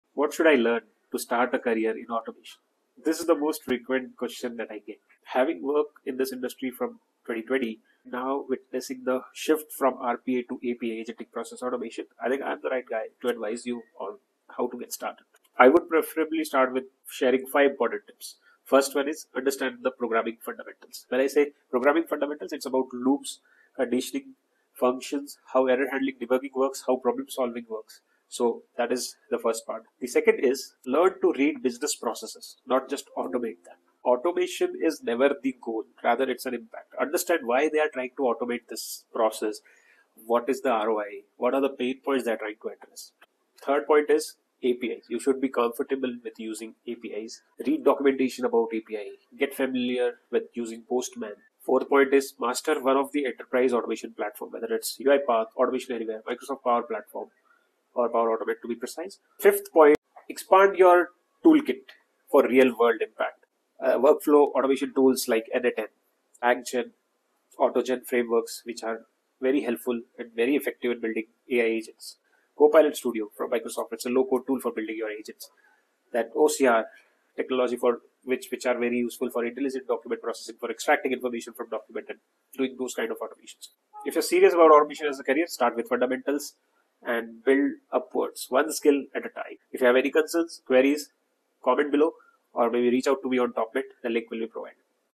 Want to start & make career in Automation & AI?
This is the most frequently asked question I receive from early-career professionals to experienced individuals looking to transition or upskill.